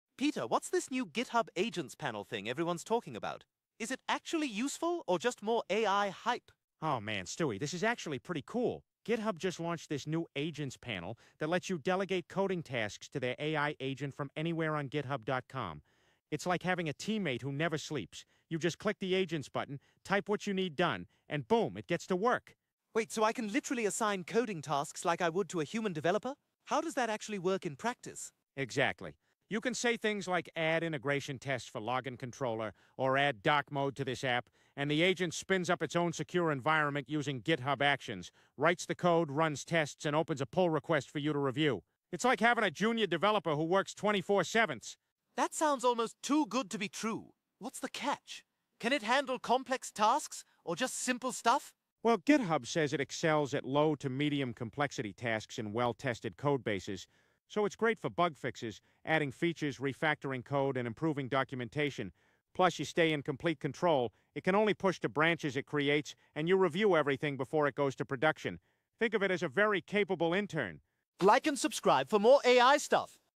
GitHub's New AI Agent Panel - Code While You Sleep!
GitHub just launched their new Agents panel that lets you delegate coding tasks to AI from anywhere on GitHub.com! Learn how this AI coding agent works, what tasks it can handle, and how it's like having a 24/7 developer teammate. Perfect for bug fixes, adding features, and handling technical debt while you focus on the creative work!